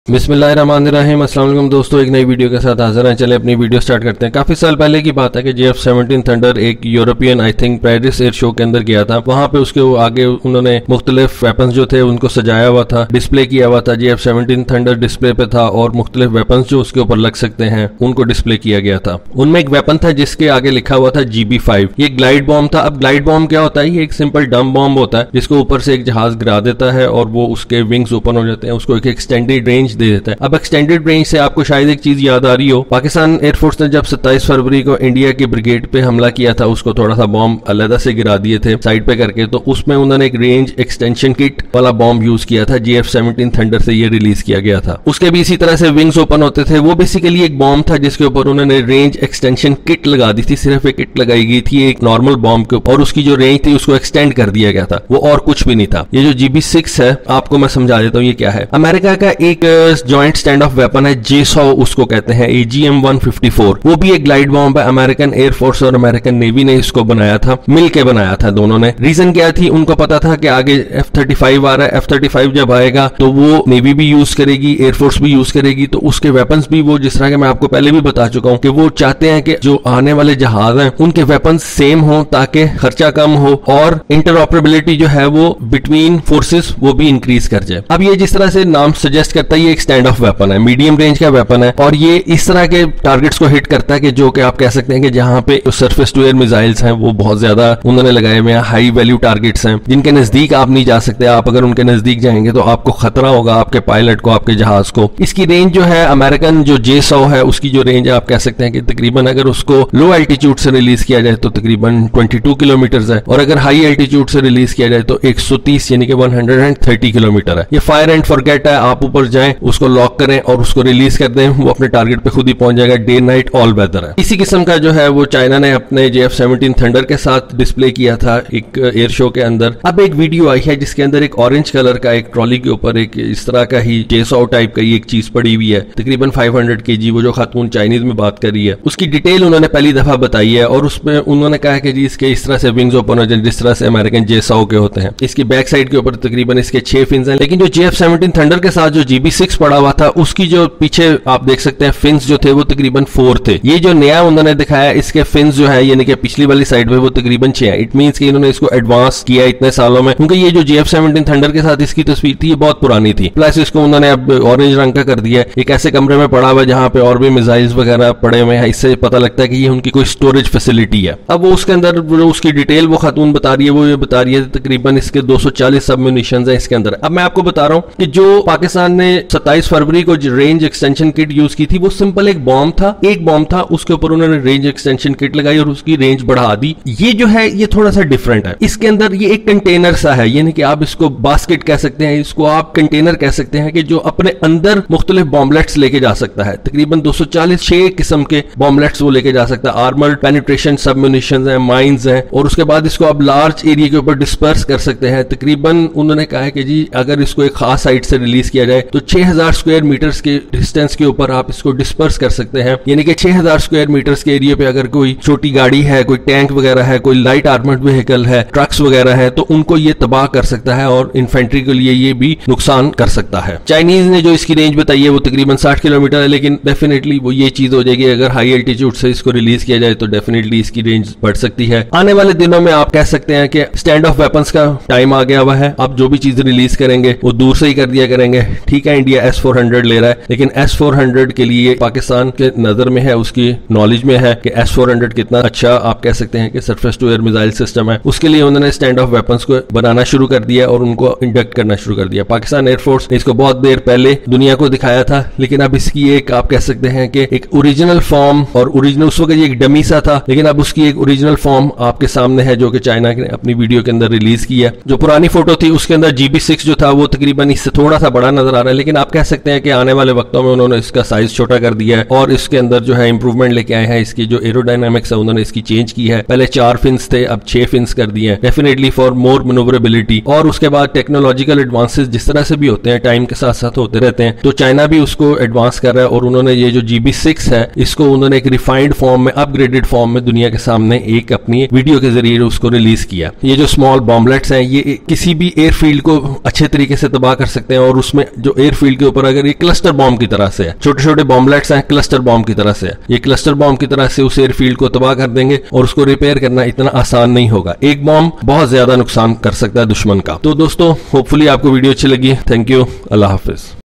GB-6 (Upgraded) for JF-17 Thunder. Looks like American AGM-164 JSOW. Standoff & safe from S-400.
1: Chinese bomb emerges in a video on internet. It’s a Glide bomb similar to American JSOW
1: AGM-154 Joint Standoff Weapon (JSOW) is a glide bomb, made jointly by USAF and USN.
2: As Name Suggests, it’s a Standoff Weapon. Medium range Precision guided Glide bomb. Specially for highly defended targets that increase aircraft survivability.
3: Range is between 22km (Low altitude release) to 130km (High altitude release).
4: JSOW is a fire and forget, Day and night, all weather.

2: Orange coloured on a trolly in a storage facility of PLAAF. domestically-made 500kg-class air-launched weapon for the first time.
3: manually extending and retracting its small wings.
4: identical tail configuration with six small fins, front end of the Chinese weapon appears less sophisticated. sleeker nose for Stealth. Low observability.
5: can release hundreds of submunitions, can effectively paralyze an airfield in one shot
6: 240 sub-munitions of six types, armor-penetrating submunitions and mines that can be dispersed across a large area, such as an airfield or a critical choke point.
7: range of more than 60 km, high strike accuracy, loaded with a variety of ammunition.
8: dispersal area for the sub-munitions as being “more than 6,000 square meters.
9: high-altitude long-range launch, autonomous all-weather attacks, and high-precision hits.
10: disable an airfield for an extended period with only one shot because the sheer number of sub-munitions means the whole runway will be destroyed, and it is also possible that some of the submunitions will be mines, which will make attempts to repair the runway very risky
11: pop-out wings to increase its glide range, allow for a precision strike at long range, without the launch aircraft needing to enter into the heart of high-threat defense zones around a high-value target, such as an airfield
12: J-8G, adopted a SEAD role
13: JF-17 Photo. TL500/GB6 is not only larger than the new weapon, but it also features a different configuration, with a cruciform tail unit (four fins) compared to the new weapon’s six.
14: new weapon provides more evidence of China’s technological advances, that in this case could be employed to disable airbases via standoff strikes in a region already overflowing with tension.